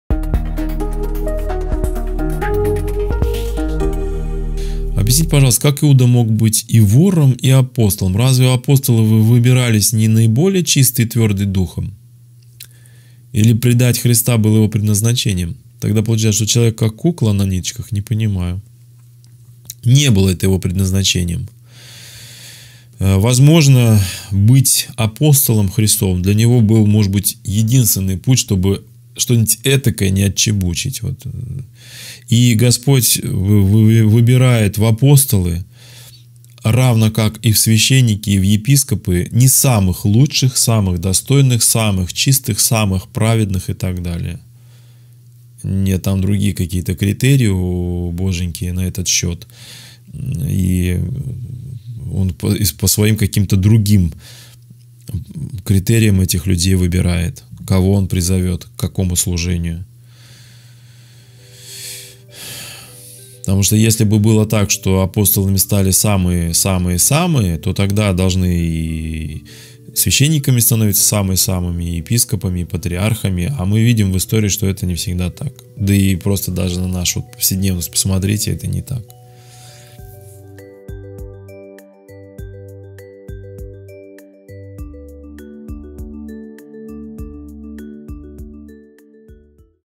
Почему Иуда стал апостолом
Описание:
Почему Иуда, не самый порядочный человек, стал апостолом?